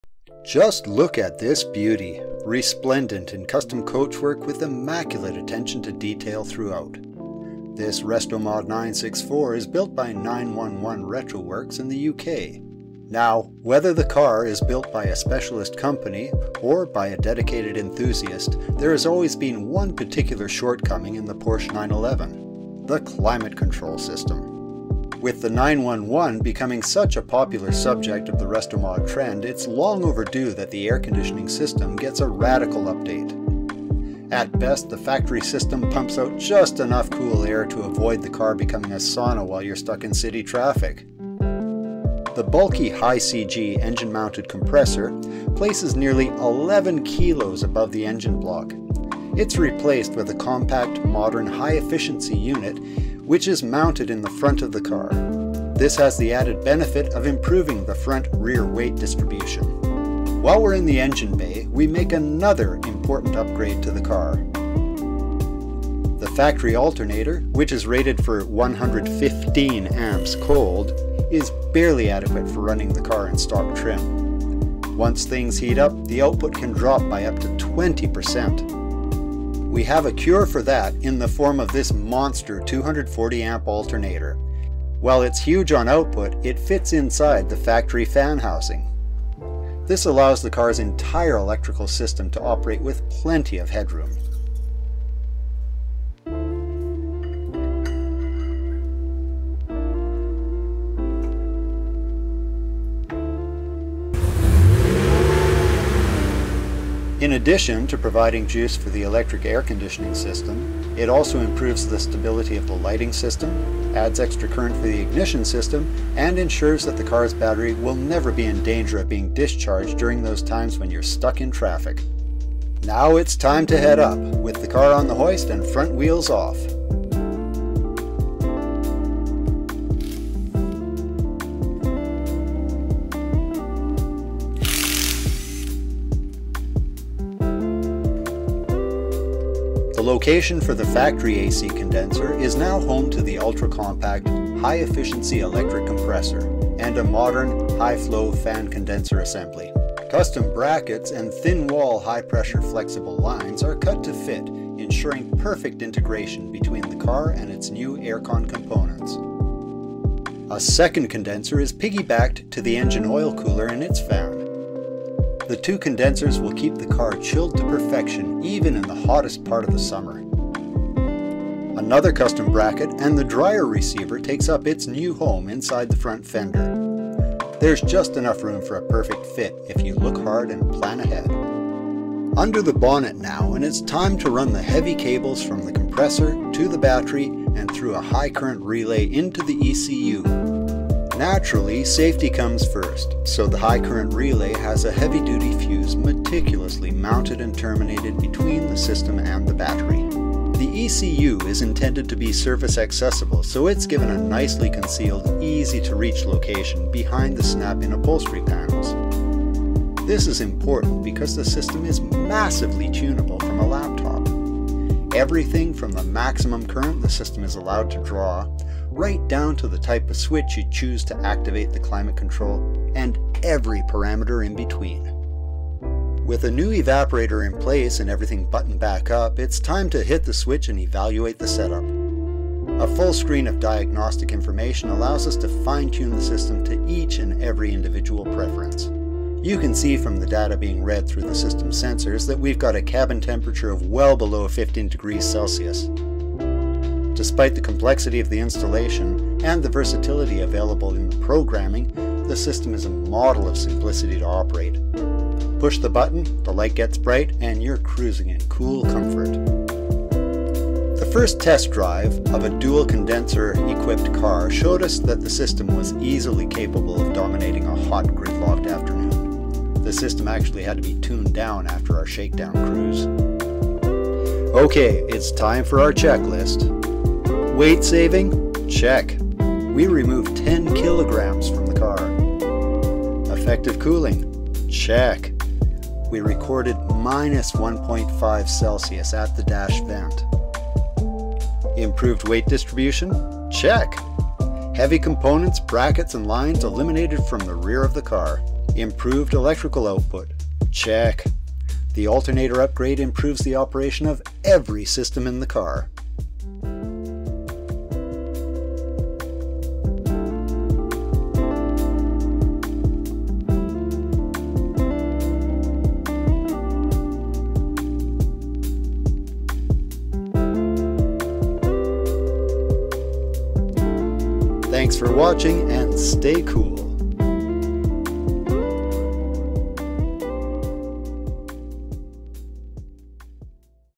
Electric Air Conditioning for Restomod Porsche 911
Installing a modern air conditioning system on a Porsche 964 Restomod.